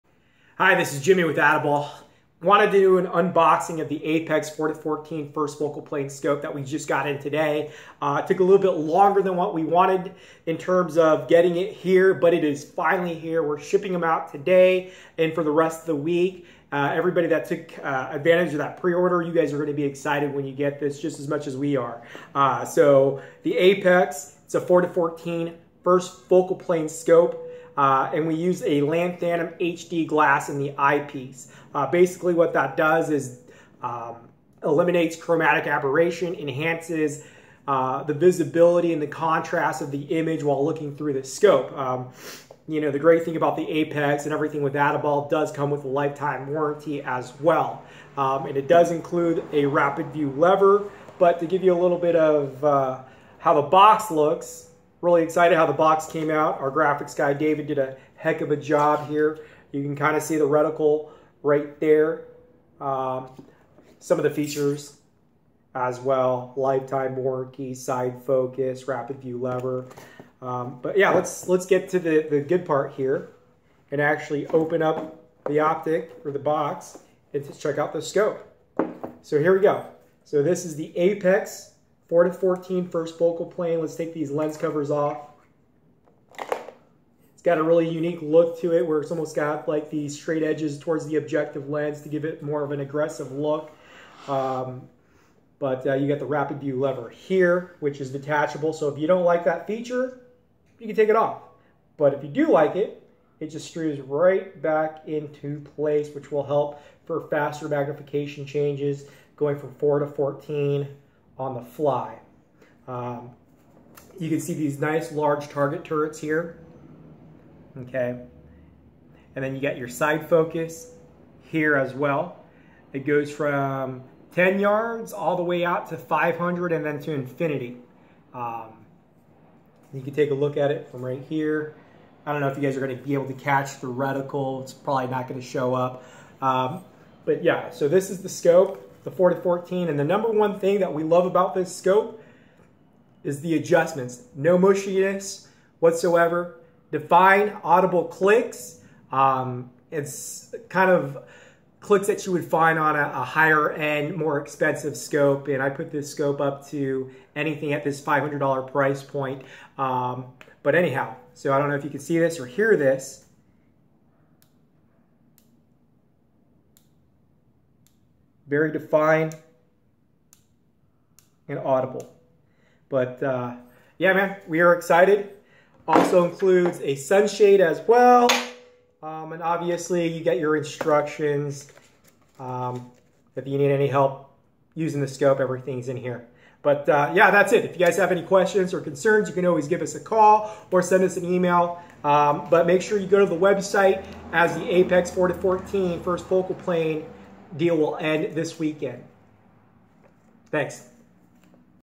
Atibal APEX 4-14x44 FFP Unboxing
We unbox the all new Atibal APEX 4-14x44 FFP scope and talk about the features.